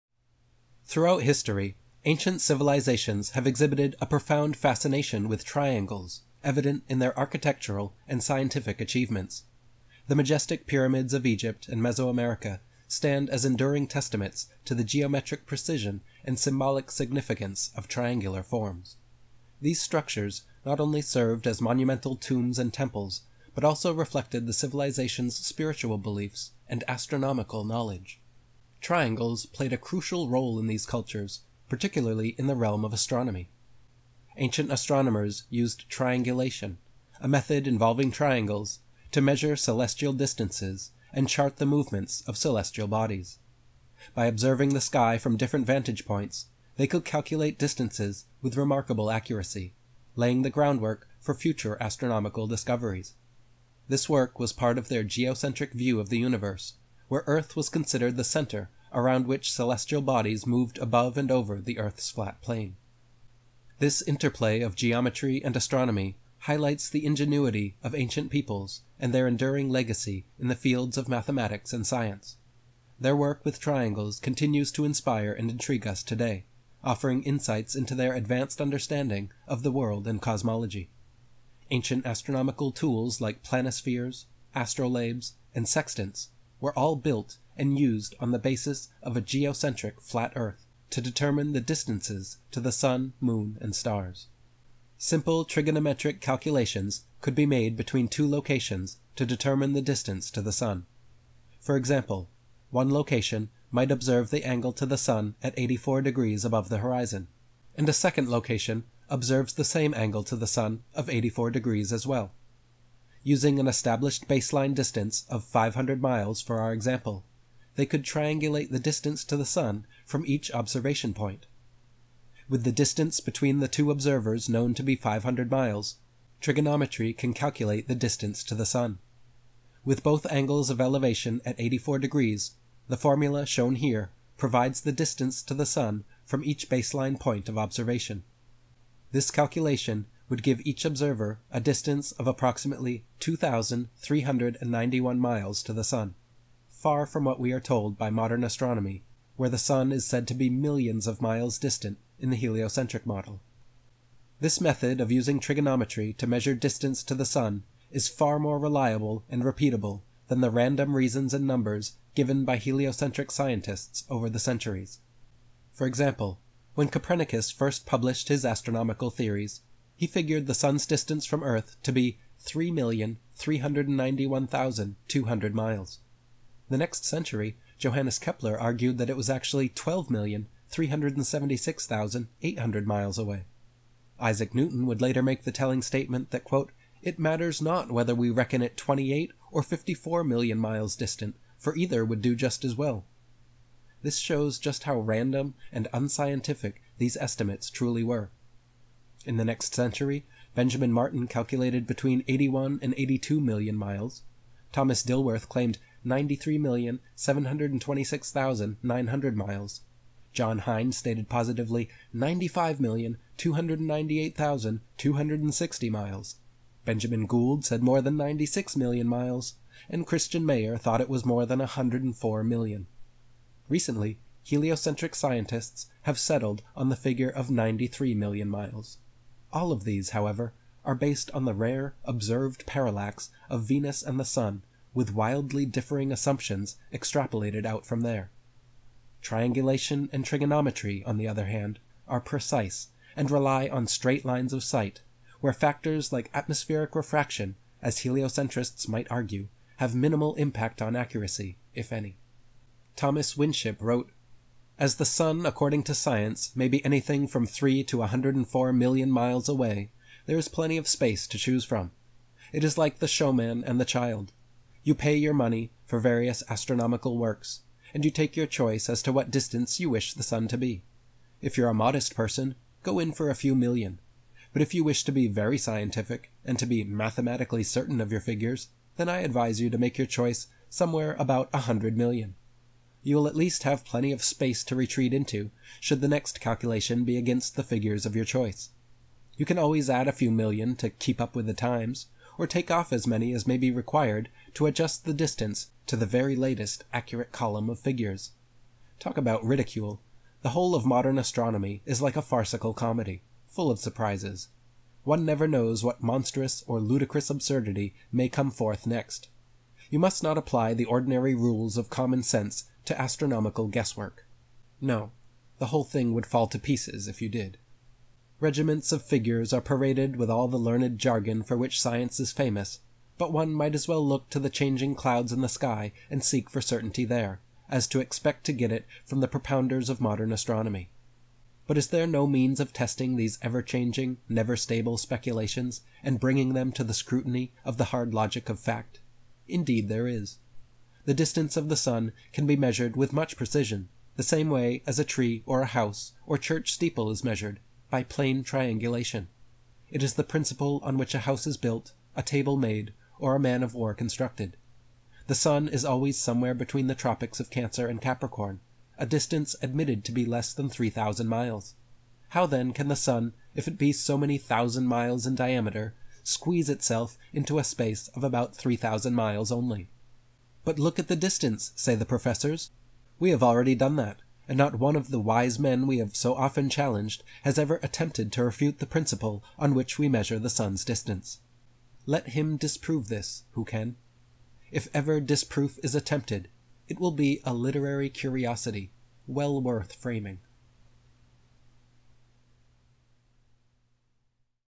How to Measure the Distance to the Sun
Ancient astronomers used triangulation, a method involving triangles, to measure celestial distances and chart the movements of celestial bodies. By observing the sky from different vantage points, they could calculate distances with remarkable accuracy, laying the groundwork for future astronomical discoveries...